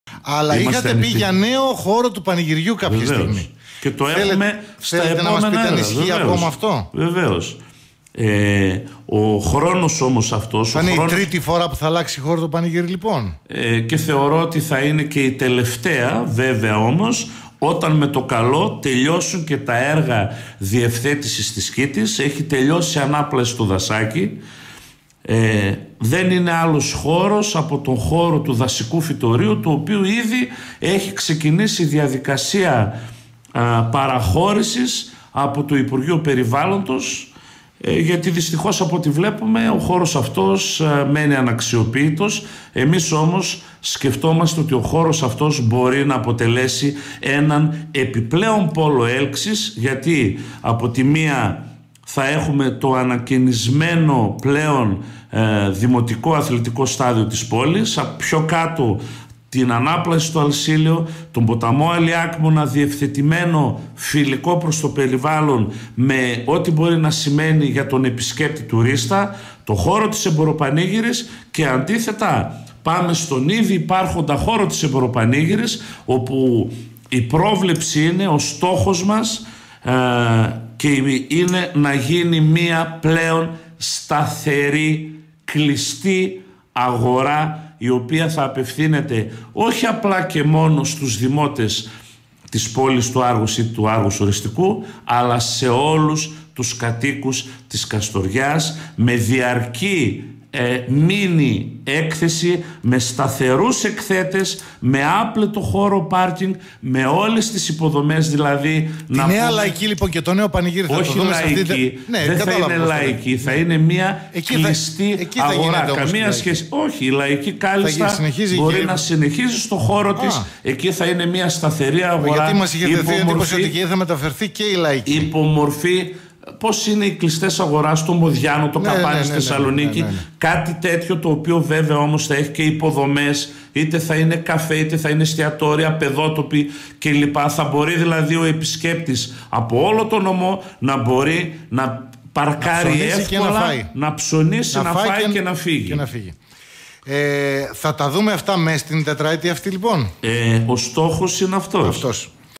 Άργος Ορεστικό: Μετακομίζει η Εμποροπανήγυρη; Δείτε τι λέει ο Π. Κεπαπτσόγλου
Τη μεταφορά της Εμποροπανήγυρης στο δασάκι του Άργους Ορεστικού προανήγγειλε ο δήμαρχος Άργους Ορεστικού, Πάνος Κεπαπτσόγλου, σε συνέντευξη του πριν λίγα λεπτά στον Χρήστο Νατσούλη και τους Antennes 93.6.

Όπως σημείωσε ο ίδιος, ο οποίος και δήλωσε πως στόχος είναι να γίνει η μεταφορά εντός της επόμενης τετραετίας: «Ο χώρος δεν είναι άλλος από αυτόν του δασικού φυτωρίου για το οποίο ήδη έχει ξεκινήσει η διαδικασία παραχώρησης από το Υπουργείο Περιβάλλοντος. Γιατί, δυστυχώς, από ότι βλέπουμε ότι ο χώρος αυτός μένει αναξιοποίητος. Εμείς σκεφτόμαστε ότι ο χώρος αυτός μπορεί να αποτελέσει έναν επιπλέον πόλο έλξης, όταν τελειώσουν και τα έργα διευθέτησης της κοίτης του Αλιάκμονα. Γιατί από τη μία θα έχουμε το ανακαινισμένο πλέον δημοτικό Αθλητικό Στάδιο της πόλης και πιο κάτω την ανάπλαση του αλσυλλίου μέ φιλικό προς το περιβάλλον με ότι μπορεί να σημαίνει για τον επισκέπτη-τουρίστα. Αντίθετα στον ήδη υπάρχοντα χώρο της εμποροπανήγυρης η πρόβλεψη και ο στόχος μας είναι να γίνει μία σταθερή κλειστή αγορά η οποία θα απευθύνεται όχι μόνο στους δημότες της πόλης του Άργους ή του δήμου αλλά σε όλους τους κατοίκους της Καστοριάς με διαρκή μίνι έκθεση με σταθερούς εκθέτες, με άπλετο χώρο parking, με όλες τις υποδομές δηλαδή».